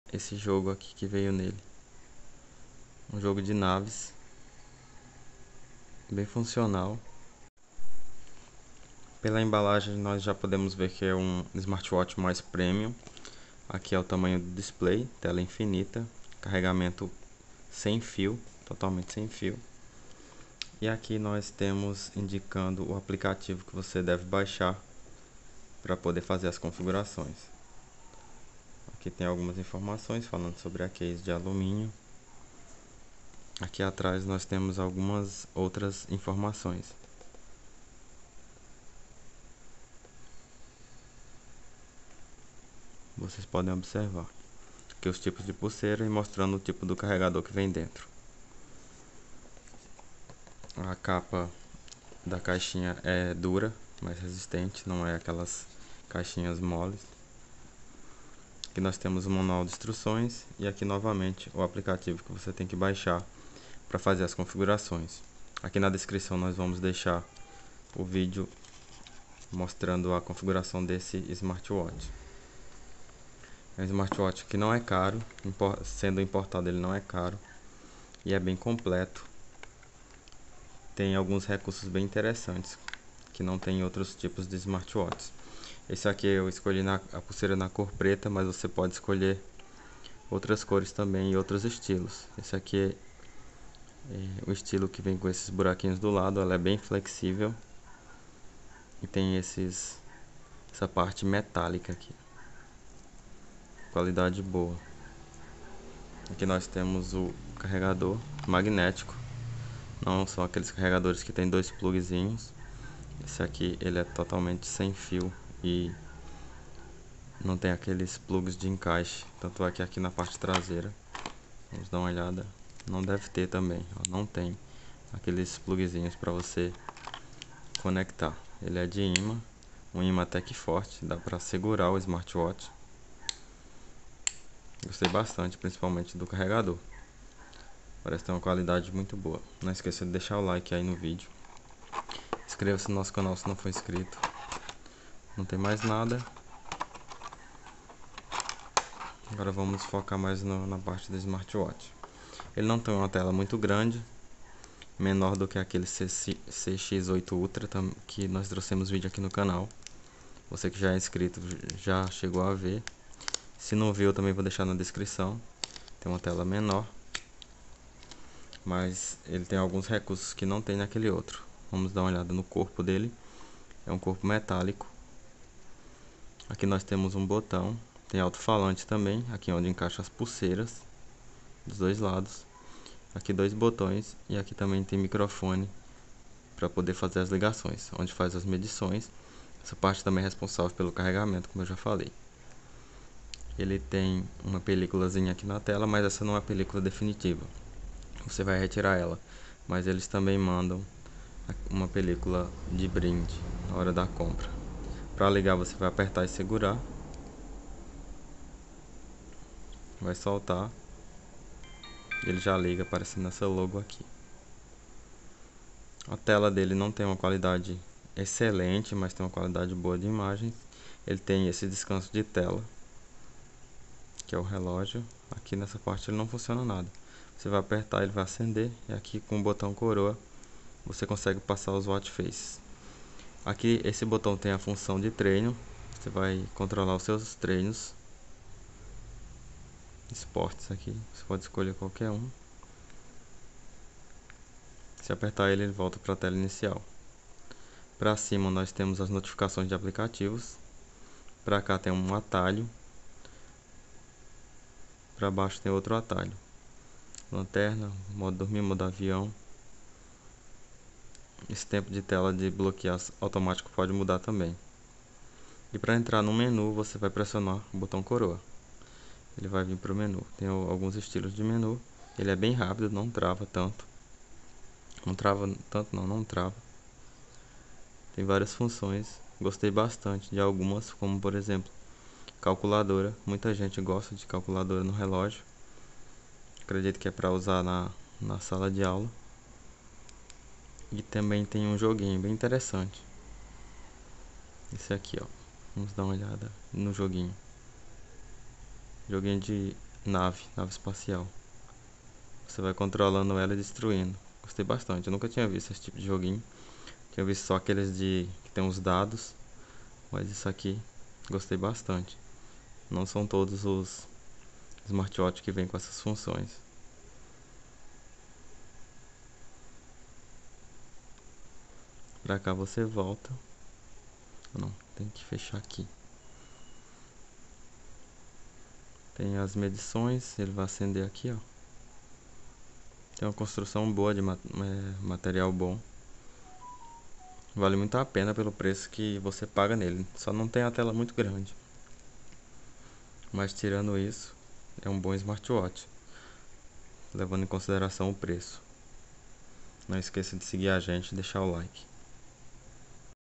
Smartwatch T800 ultra, super completo com calculadora jogo e carregador magnético, compensa?
Nesse vídeo trouxemos o Smartwatch T800 ultra, super completo com calculadora jogo e carregador magnético, compensa? vejam o vídeo Smartwatch T800 ultra, super completo com calculadora jogo e carregador magnético, compensa? e deixe seu like.